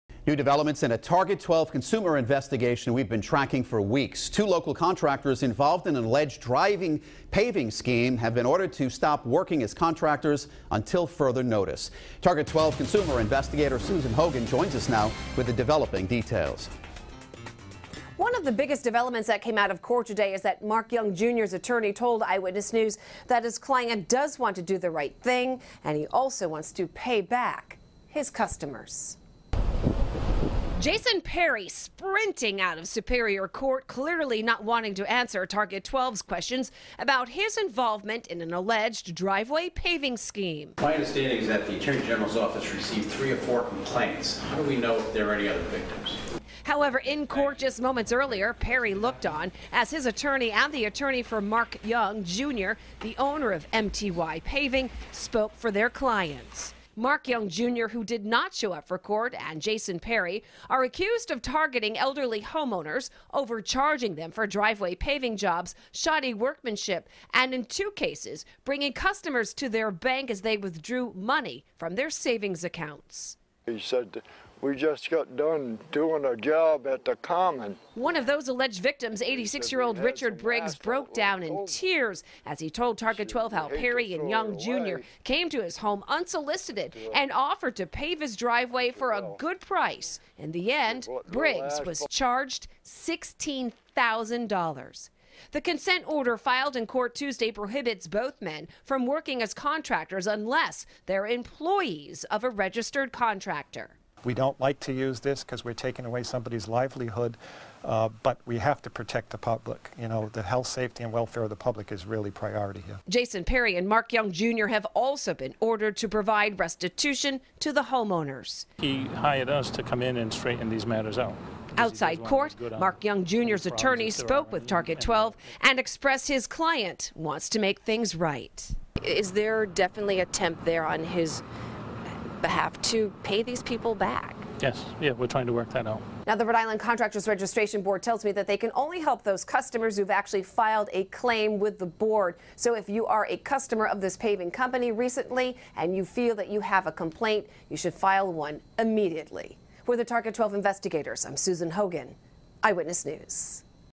Paving Company Ordered to Stop Contracting Until Further Notice
An attorney for Mark Young, Jr. said in court that his client wants to do the right thing and pay back his customers.